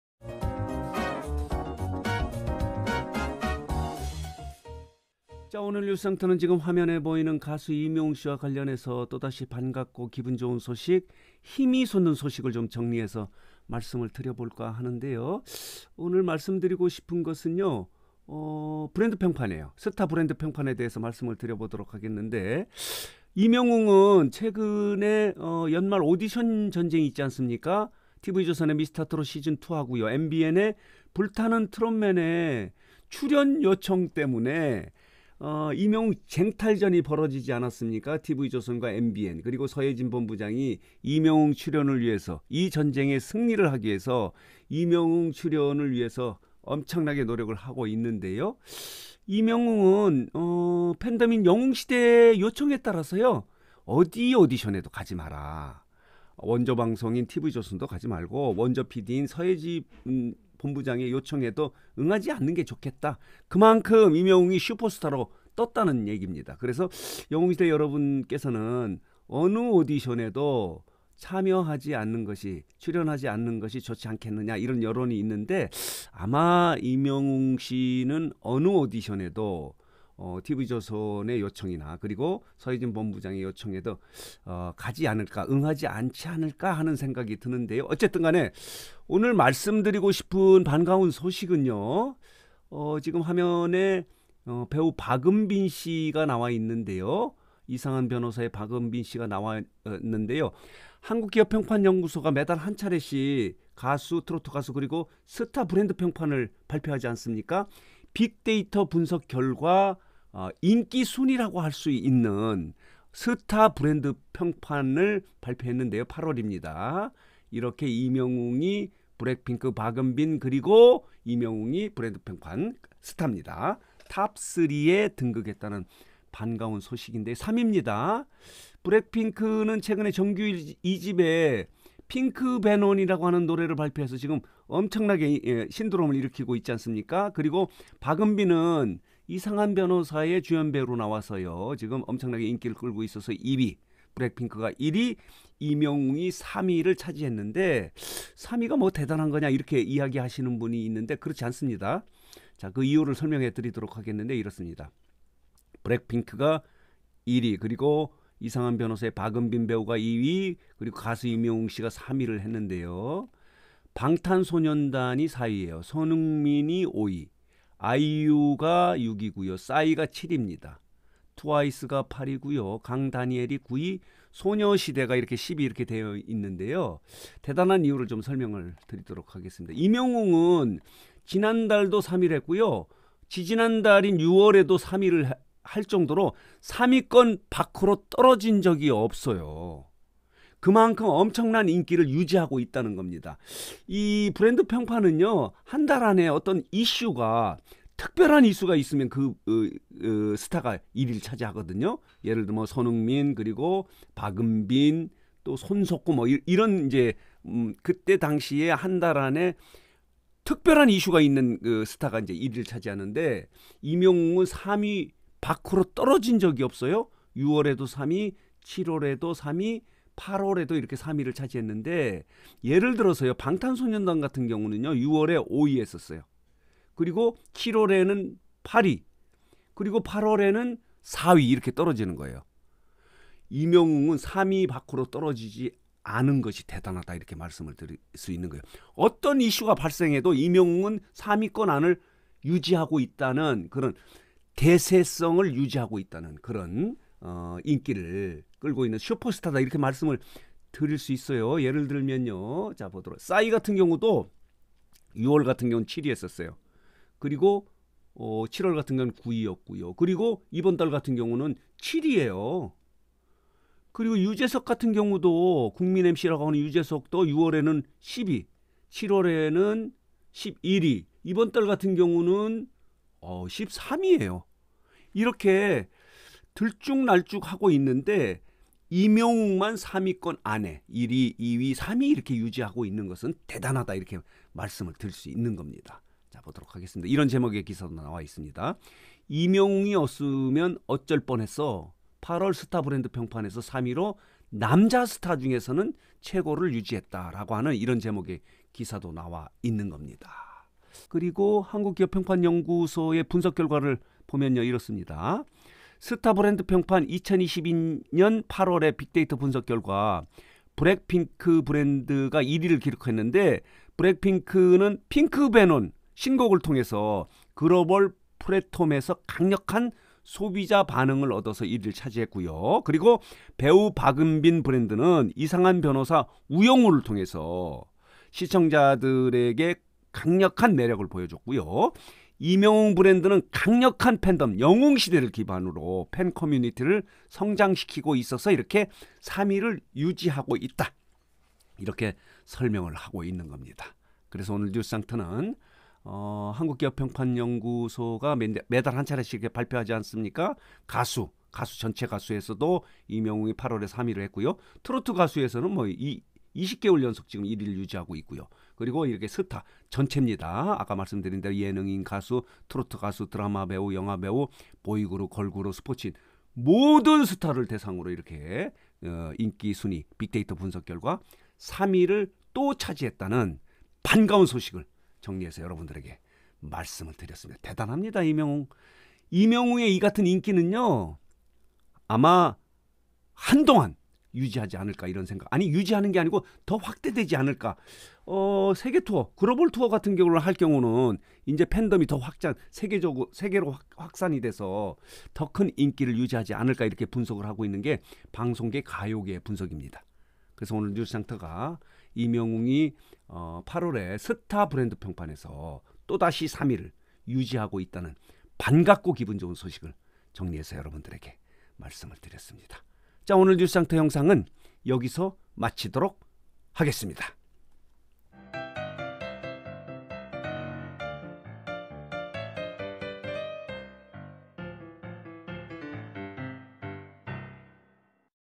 임영웅을 '독보적 최고 스타'라고 말하는 진짜이유? 어떤 이슈에도 흔들리지 않는 인기! 정규앨범 콘서트 뉴스장터
임영웅을 '독보적 최고 스타'라고 말하는 진짜이유? 어떤 이슈에도 흔들리지 않는 인기!